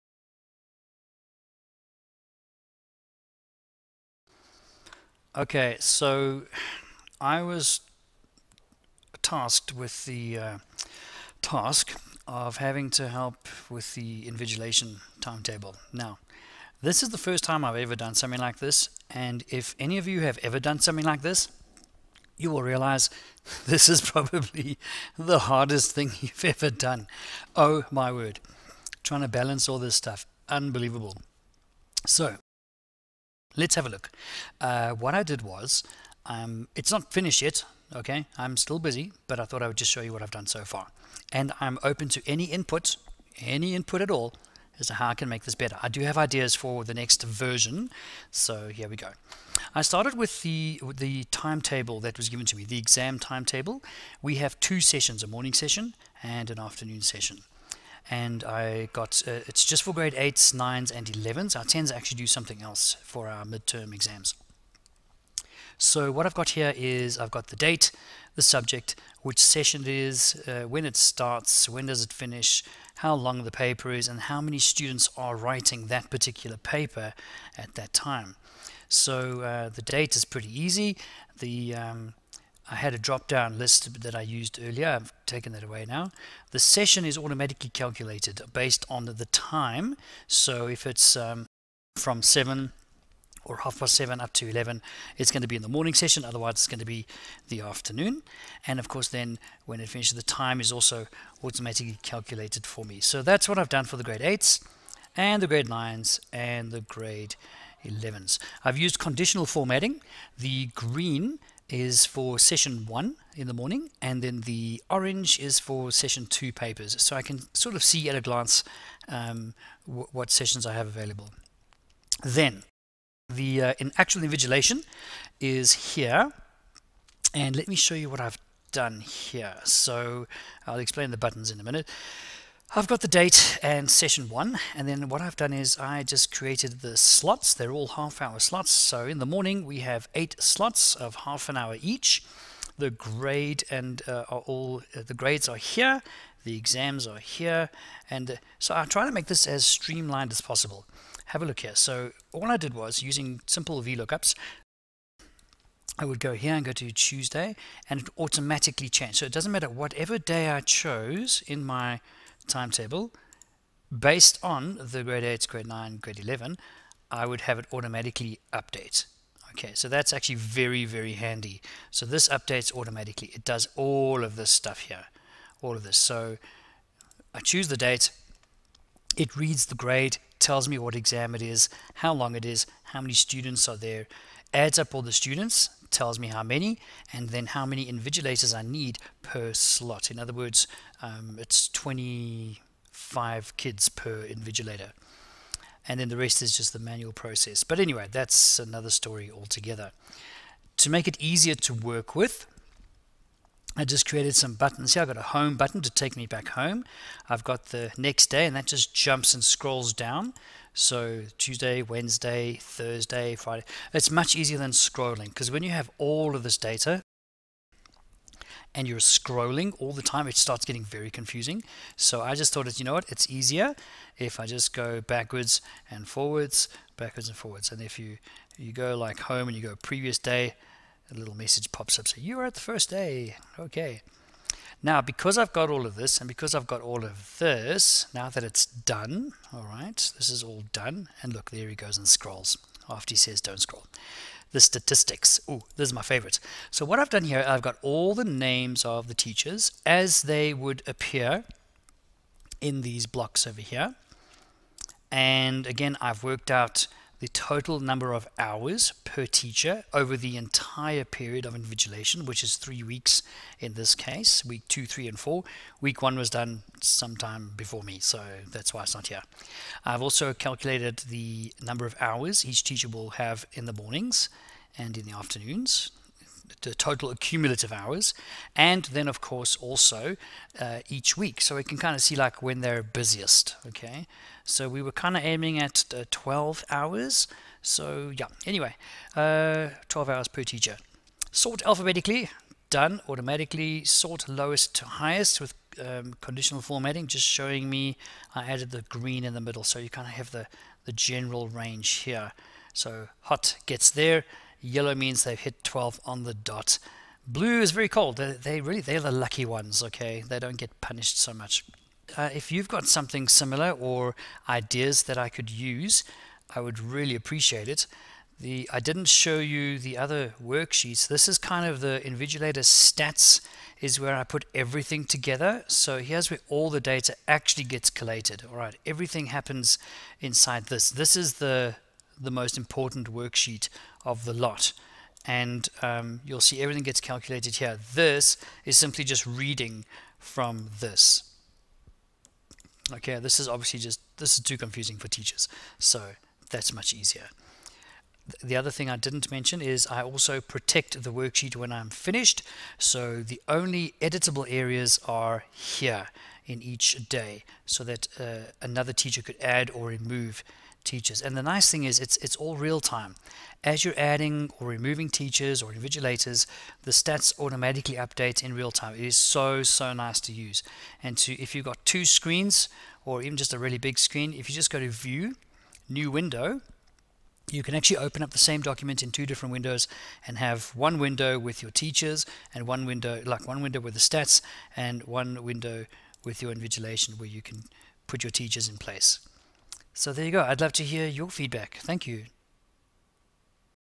Invigilation timetable
How I am using MS Excel to create a workable invigilation timetable for my school. Not perfect, but the next version will be awesome!